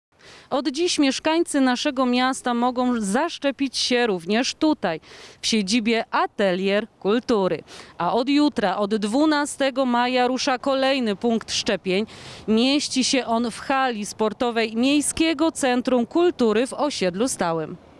Od dziś 11 maja, możemy się zaszczepić w punkcie znajdującym się w siedzibie ATElier Kultury
Właśnie dziś, 11 maja 2021 roku ruszył kolejny powszechny punkt szczepień w Jaworznie. Mieszkańcy mogą przyjąć szczepionkę w punkcie znajdującym się w siedzibie ATElier Kultury przy ul. Mickiewicza 2 (dawny Teatr Sztuk) w Centrum.

Kolejny powszechny punkt szczepień rusza już jutro, 12 maja, zlokalizowany jest w Hali Sportowej w Osiedlu Stałym, przy ul. Inwalidów Wojennych – organizowany przez podmiot prywatny – Pro4Med Sp. z o.o.

Punkt w siedzibie ATElier Kultury będzie czynny w dni robocze w godzinach:

11 – 12 maja 2021 r. w godz. 8:00 – 11:00,
13 – 14 maja 2021 r. w godz. 8:00-12:00,
od 18 maja 2021 r. w godz. 8:00 – 13:30.

Przypominamy, że rejestracja do punktu możliwa jest przez:

sms-owa – nr telefonu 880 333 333.

Zarejestrować i zaszczepić mogą się również osoby, które nie skorzystały ze szczepień w pierwszym kwartale tego roku w ramach dawek dla poszczególnych grup. Wystarczy wyrazić chęć szczepienia jedną z powyższych metod.